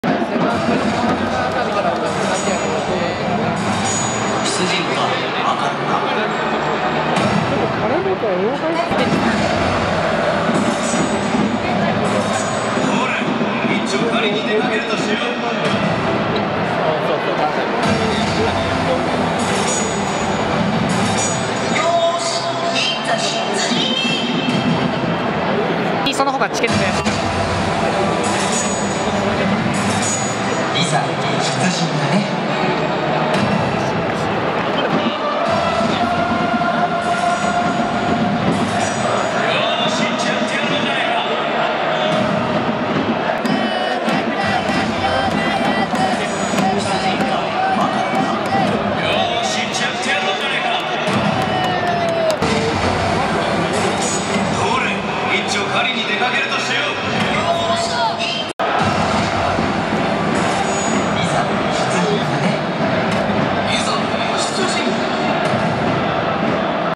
ミュージカル「刀剣乱舞」コラボナイター 攻撃開始前ジングルまとめ
2018年9月11日に開催された「刀剣乱舞」コラボナイターでの巨人攻撃開始前に流れた限定ジングルのまとめ。(一部抜け落ちあり)